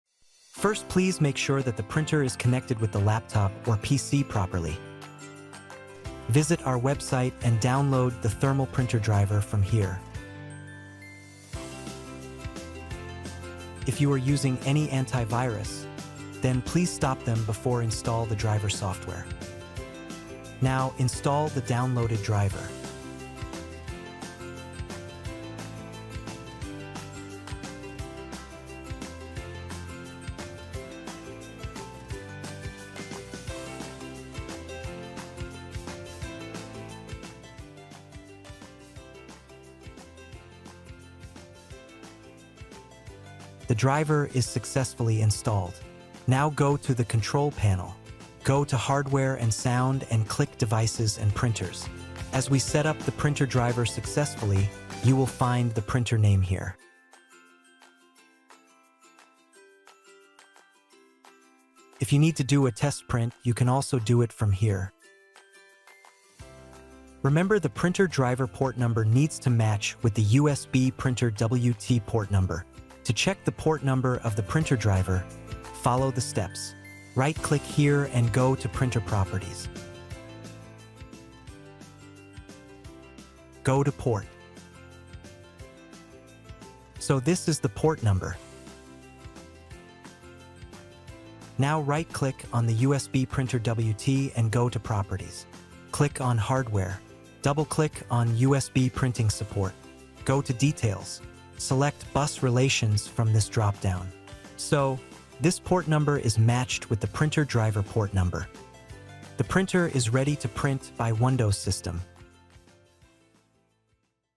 How to Connect Printer with Windows System | Grozziie
In this video we have use TP876 Model printer, the instruction is same for our TP518, TP870, TP870Plus, TP876Plus, GZP820 Model printer.